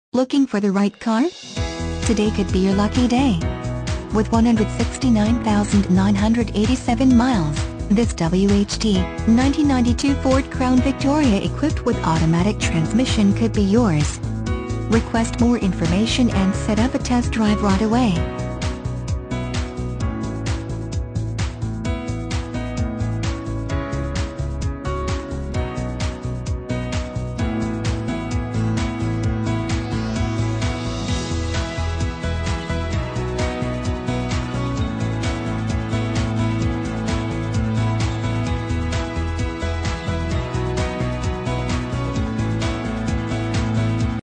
1992 FORD CROWN VICTORIA Council Bluffs, IA 2259B
1992 FORD CROWN VICTORIA Council Bluffs, IA
Stock

Don't miss this 1992 Ford Crown Victoria. It's equipped with automatic transmission and features a WHT exterior. With 169987 miles, you'll want to take this car home. Make a great choice today.